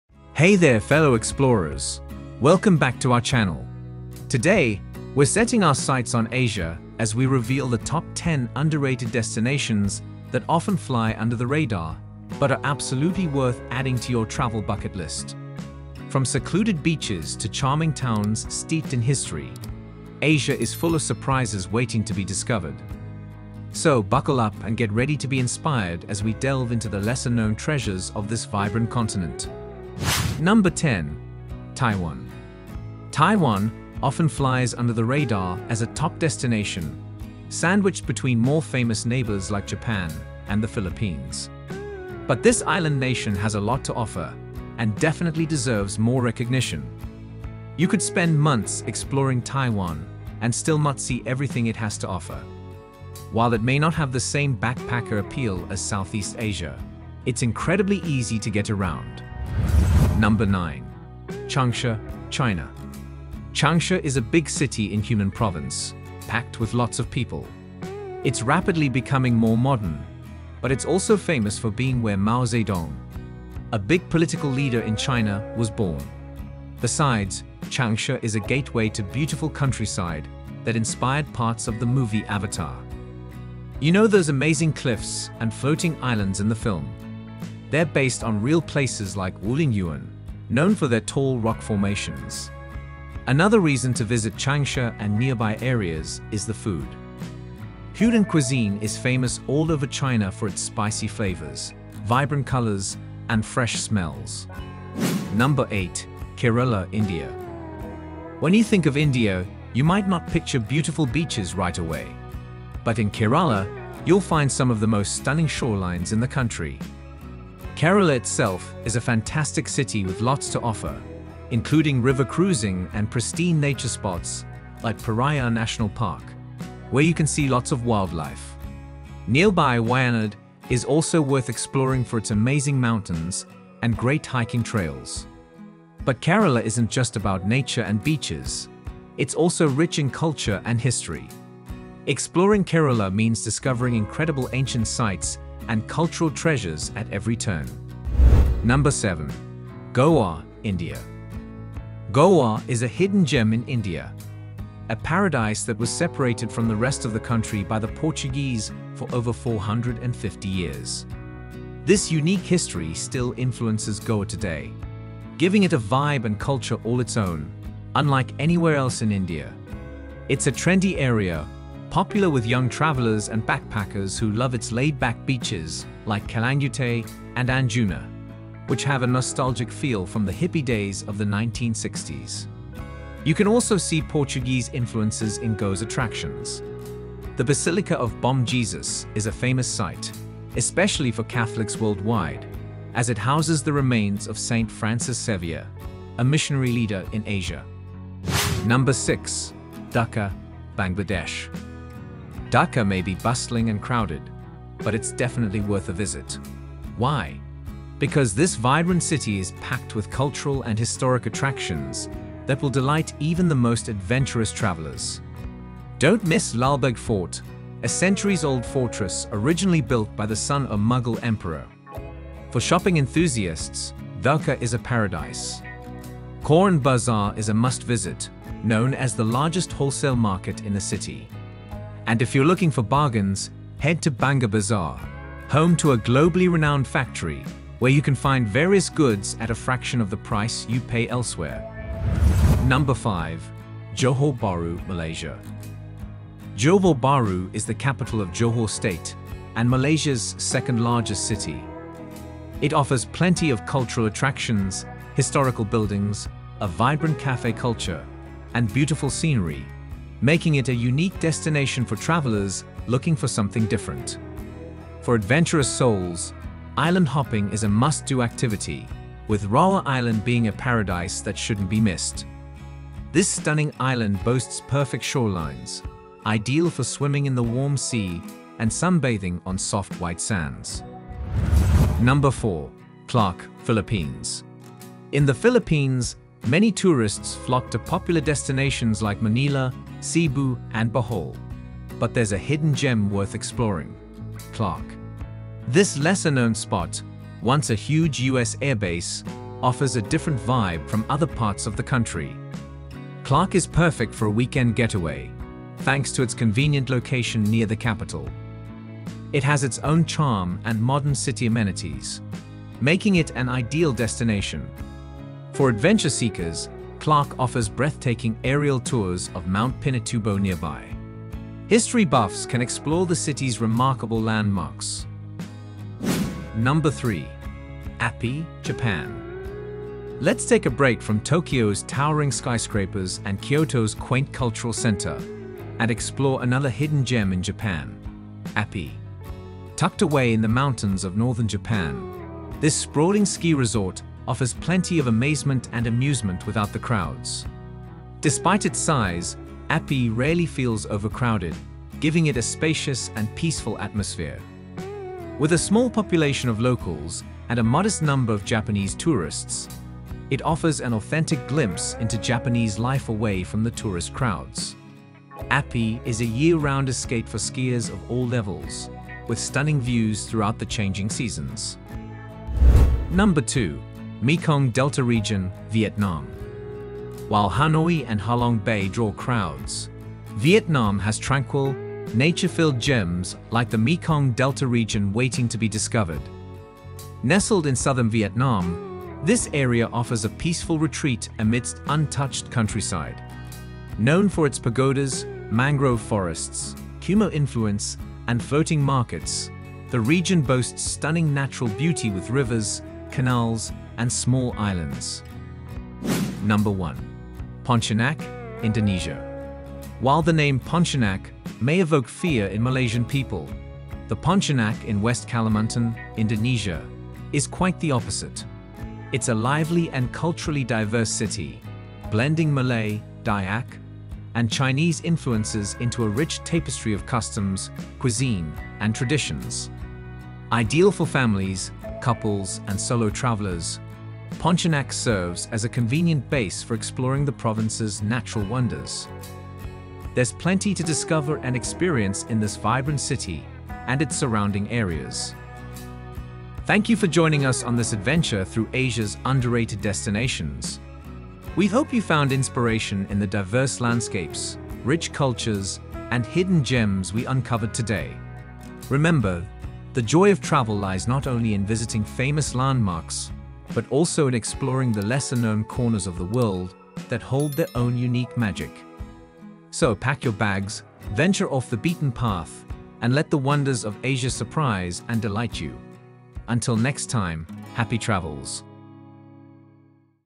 Secrets Revealed: The Ultimate Crowd-Free ASIA Vacation Guide
Asia is home to some of the most breathtaking Crowd-Free Vacation Spots? The most breathtaking GEMS and TOP underrated destinations in Asia that will change your travel bucket list forever! These 10 hidden gems will surely surprise and delight you. From remote islands to ancient temples, get ready to be inspired to explore beyond the typical tourist spots. Watch now and start planning your next unforgettable adventure in Asia.

Discovering lesser-known destinations in Asia can be a challenge when popular spots often steal the spotlight. We'll take you on a journey to uncover unique and off-the-beaten-path locations that will leave you in awe.

Get ready for the moment where we reveal the top hidden gem that will make you want to pack your bags immediately!

Let's inspire each other to explore the world and create unforgettable memories.

Chapters:
0:00 - Intro
0:30 - Taiwan
1:00 - Changsha, China
1:45 - Kerala, India
2:28 - Goa, India
3:21 - Dhaka, Bangladesh
4:04 - Johor Bahru, Malaysia
4:42 - Clark, Philippines
5:28 - Appi, Japan
6:12 - Mekong Delta Region, Vietnam
6:47 - Pontianak, Indonesia
7:27 - Outro